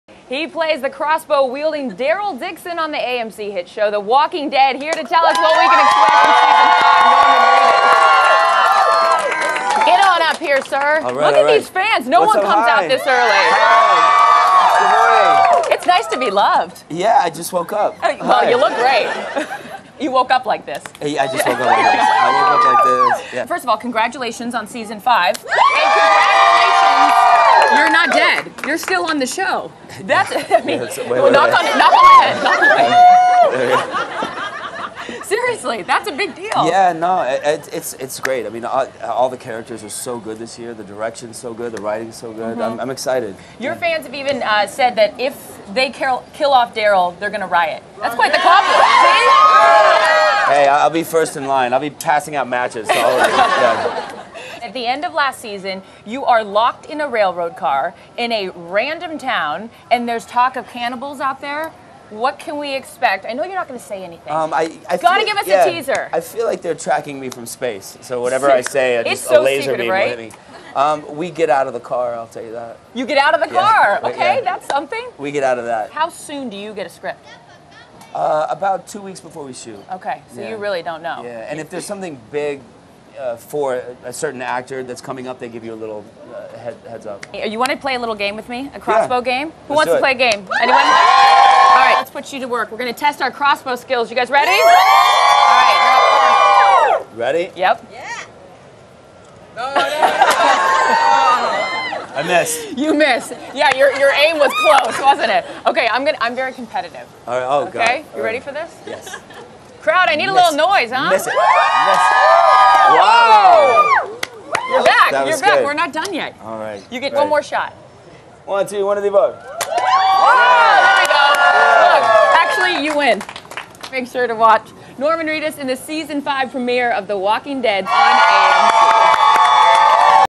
Norman Reedus, aka Daryl, Drops Hint About Season 5 of ‘The Walking Dead’
Actor Norman Reedus, who plays badass Daryl Dixon on the hit AMC show “The Walking Dead,” joined “Extra’s” Charissa Thomspon at Universal Studios Hollywood and talked about the upcoming Season 5.